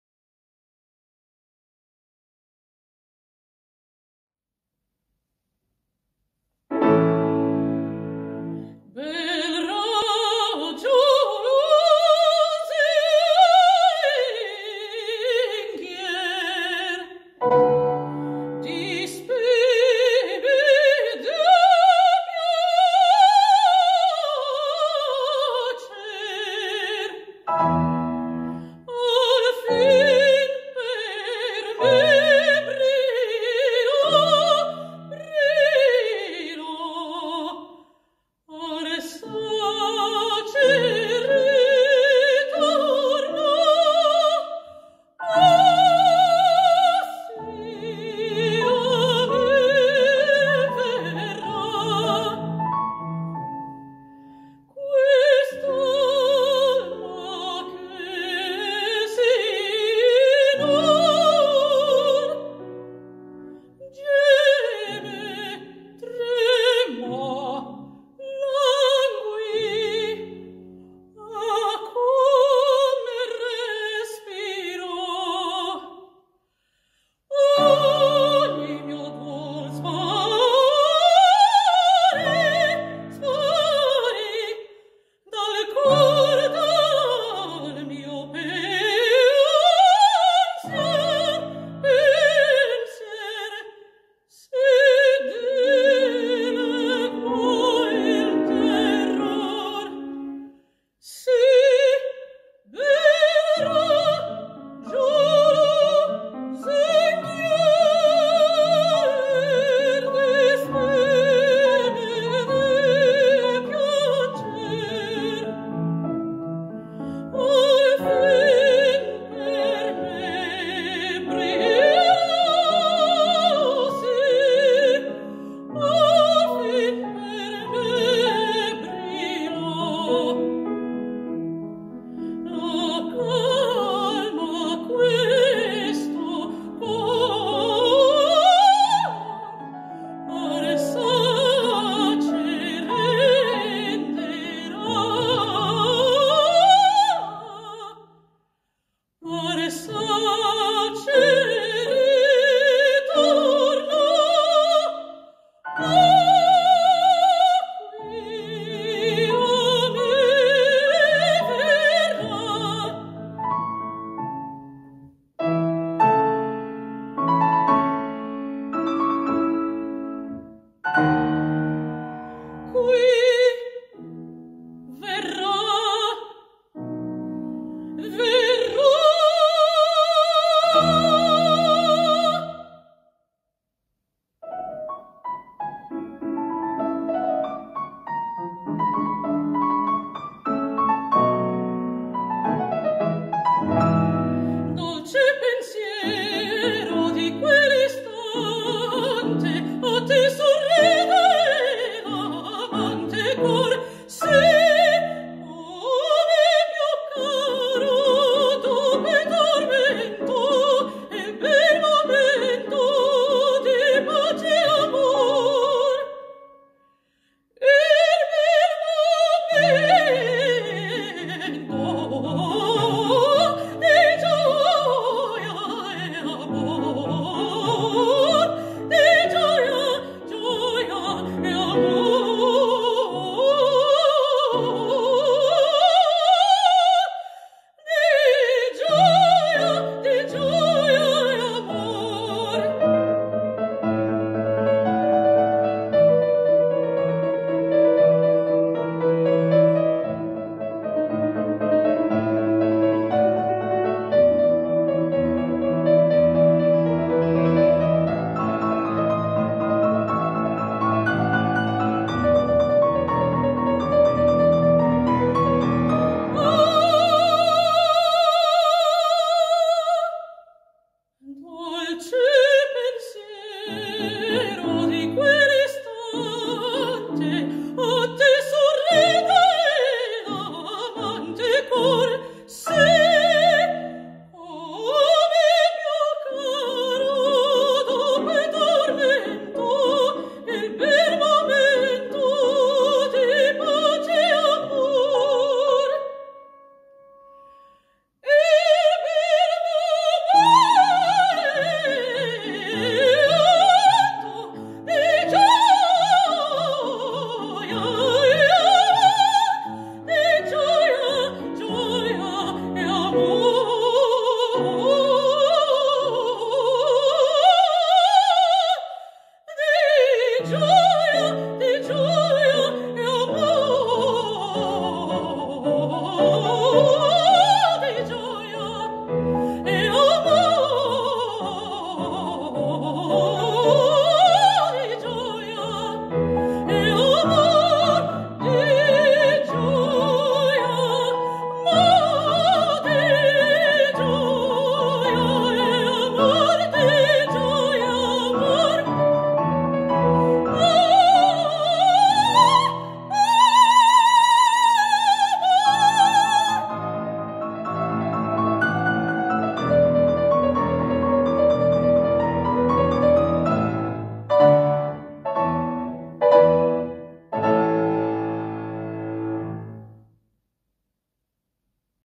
Nouvelles Étoiles 6 - Vocals - Beatriz Pereira - 1st prize
1st Prize laureates of Nouvelles Étoiles - Online International Music Competition - 6th Edition, in the Vocals category: Beatriz Pereira (from Portugal)